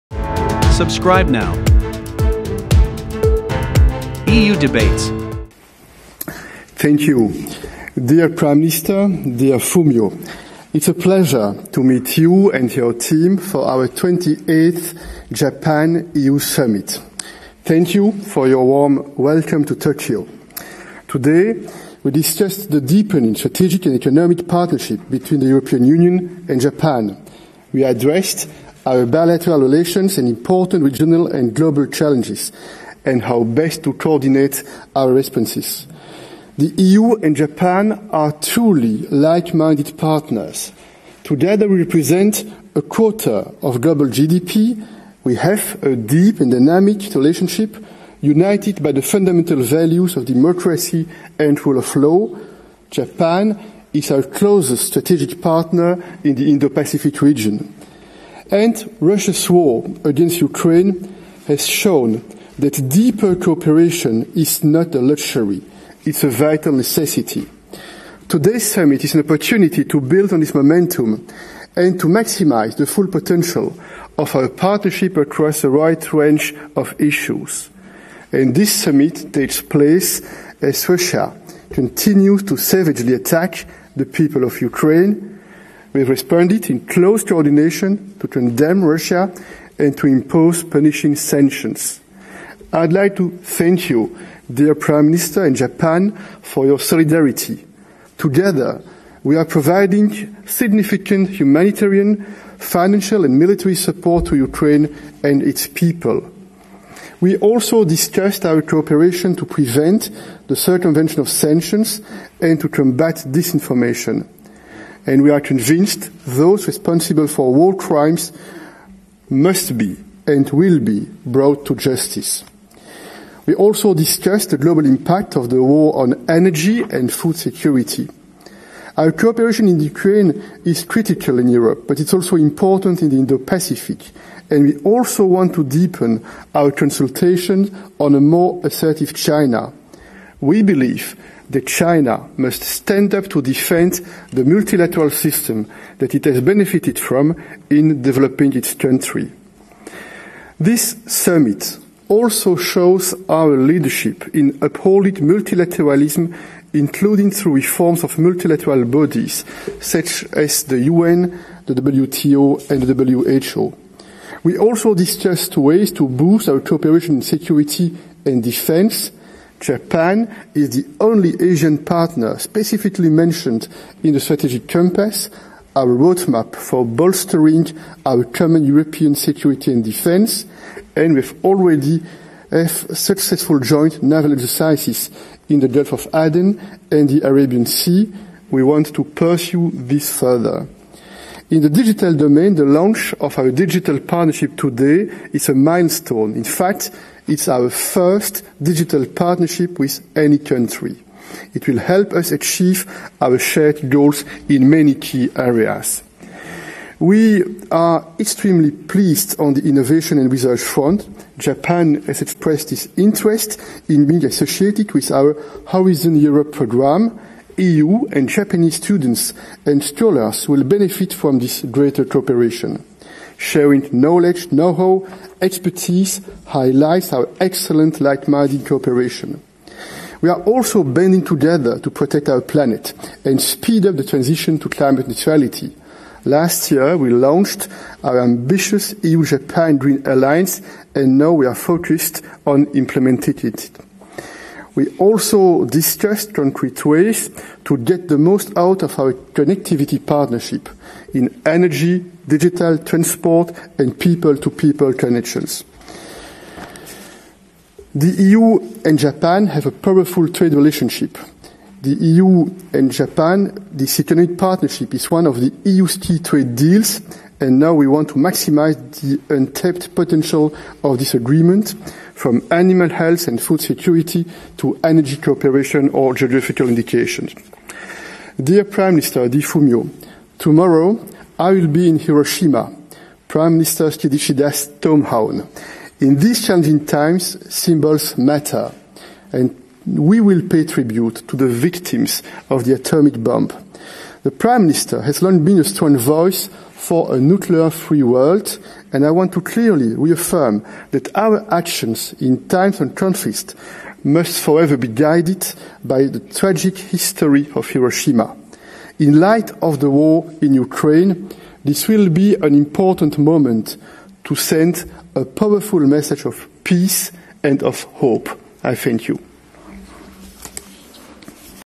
Charles Michel, President of the European Council debates in EU-Japan Summit, Tokyo
Dear prime minister, dear Fumio, It’s a pleasure to meet you and your team for our 28th Japan-EU Summit. Thank you for your warm welcome to Tokyo.

Today we discussed the deepening strategic and economic partnership between the European Union and Japan. We addressed our bilateral relations and important regional and global challenges, and how best to coordinate our responses.

eudebates the unique initiative aiming to promote debate, dialogue, knowledge, participation and communication among citizens.

The EU and Japan are truly like-minded partners. Together we represent a quarter of global GDP. We have a deep and dynamic relationship, united by the fundamental values of democracy and rule of law. Japan is our closest strategic partner in the Indo-Pacific region.

And Russia’s war against Ukraine has shown that deeper cooperation is not a luxury, it’s a vital necessity. Today’s summit was an opportunity to build on this momentum. And to maximise the full potential of our partnership across a wide range of issues.

This summit takes place as Russia continues to savagely attack the people of Ukraine. We have responded in close coordination, to condemn Russia and to impose punishing sanctions.

I would like to thank you, dear prime minister, and Japan, for your solidarity. Together, we are providing significant humanitarian, financial and military support to Ukraine and its people.

We also discussed our cooperation to prevent the circumvention of sanctions and to combat disinformation. And we are convinced that those responsible for war crimes must be and will be brought to justice. We also discussed the global impact of the war on energy and food security.

Our cooperation in Ukraine is critical in Europe, but it’s also important in the Indo-Pacific, and we also want to deepen our consultations on a more assertive China. We believe that China must stand up to defend the multilateral system that it has benefited from in developing its country.

This summit also shows our leadership in upholding multilateralism, including through reforms of multilateral bodies, such as the UN, the WTO and WHO.

We also discussed ways to boost our cooperation in security and defence. Japan is the only Asian partner specifically mentioned in the Strategic Compass, our roadmap for bolstering our common European security and defence. And we have already successfully conducted joint naval exercises in the Gulf of Aden and the Arabian Sea; we want to pursue this further.

In the digital domain, the launch of our digital partnership today is a milestone. In fact, it’s our first digital partnership with any country. It will help us achieve our shared goals in many key areas.

We are extremely pleased on the innovation and research front. Japan has expressed its interest in being associated with our Horizon Europe programme. EU and Japanese students and scholars will benefit from this greater cooperation. Sharing knowledge, know-how and expertise highlights our excellent like-minded cooperation.

We are also banding together to protect our planet and speed up the transition to climate neutrality. Last year we launched our ambitious EU-Japan Green Alliance and now we are focused on implementing it.

We also discussed concrete ways to get the most out of our connectivity partnership, in energy, digital, transport and people-to-people connections.

The EU and Japan have a powerful trade relationship. The EU’s and Japan’s economic partnership is one of the EU’s key trade deals. And now, we want to maximise the untapped potential of this agreement, from animal health and food security to energy cooperation and geographical indications.

Tomorrow, I will be in Hiroshima, prime minister Kishida’s home town. In these challenging times, symbols matter. We will pay tribute to the victims of the atomic bomb. The prime minister has long been a strong voice for a nuclear-free world. And I want to clearly reaffirm that our actions, in times of conflict, must for ever be guided by the tragic history of Hiroshima. In the light of the war in Ukraine, this will be an important moment to send a powerful message of peace and of hope.